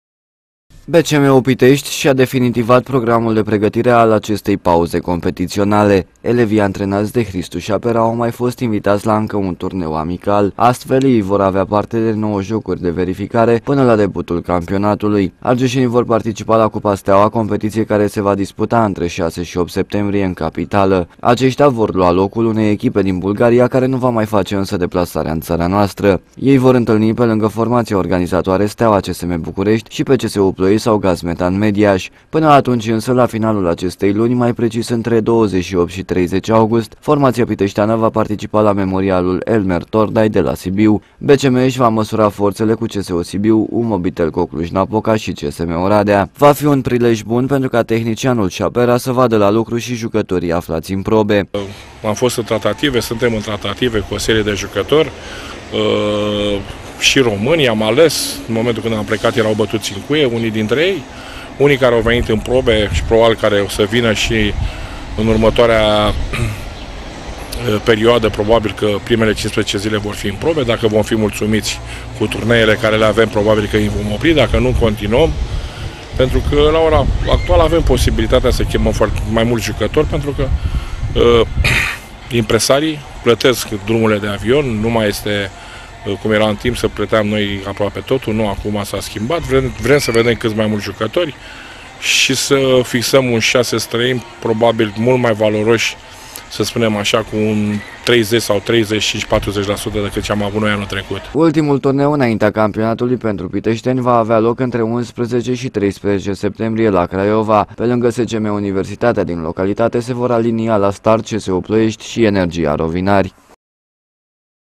BCM Pitestui va participa la trei turnee amicale.
BCM Pitestui va participa la trei turnee amicale. hristu sapera isi va testa elevii in cadrul a 9 jocuri de verificare.